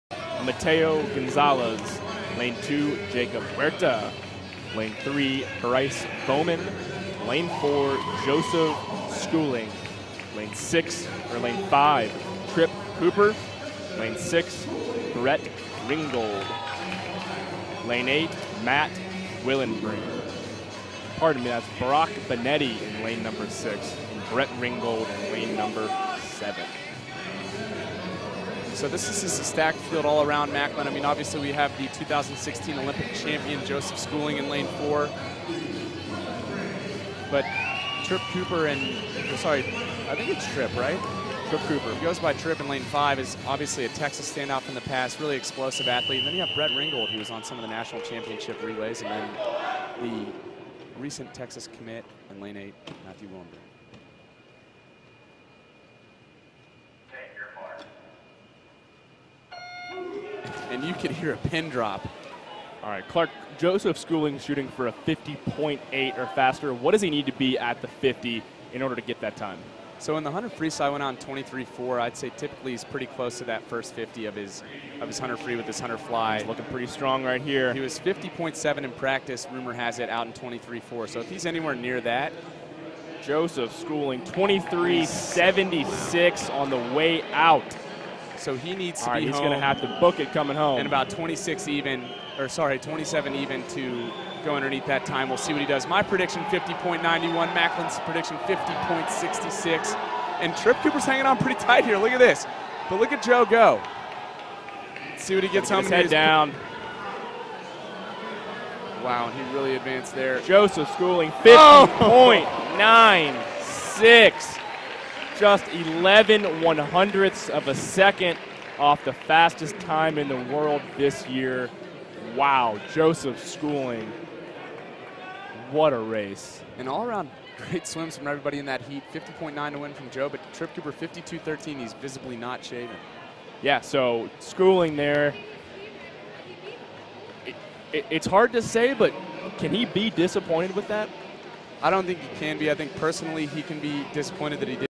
Joseph Schooling's 50.96 100m Butterfly
Joseph Schooling ​dropped a very swift 50.96 in the 100m butterfly Saturday night in Austin -- the second-fastest time in the world this year. The 2016 Olympic gold medalist was looking to get back to top form and his performance Saturday night was a strong indicator he has achieved that.

2017 Top 5 World Rankings -- Men's 100m Butterfly
1.) Caeleb Dressel (USA) -- 50.87
2.) Joseph Schooling (Singapore) -- 50.96
3.) Chad le Clos (South Africa) -- 51.29
4.) Tim Phillips (USA) -- 51.30
5.) Jack Conger (USA) -- 51.33

Watch the race video in its entirety above.